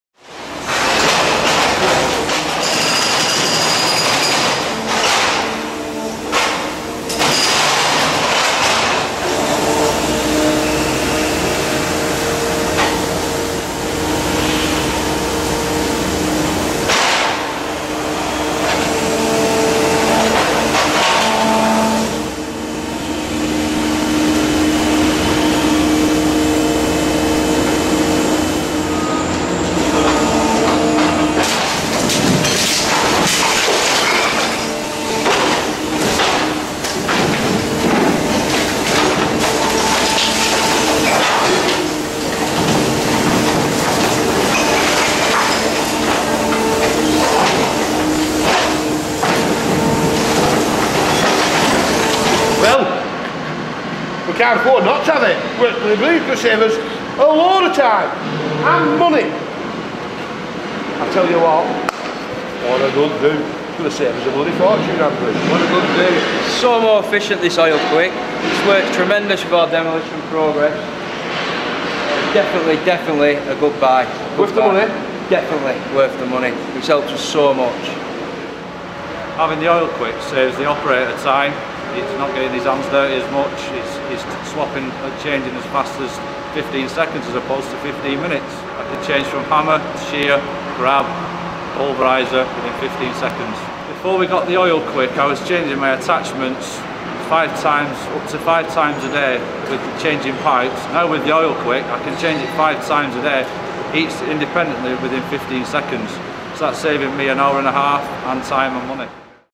Bradley Demolition Convert Brokk to OilQuick
We approached attachment specialists, ECY Haulmark for a unique and bespoke engineering solution to adapt the OilQuick system to suit our Brokk remote controlled demolition machine. This is the first such installation for the UK demolition industry.

We recently purchased a Brokk 160 model to assist with the internal soft stripping within the confined areas on our current project at Manchester Business School.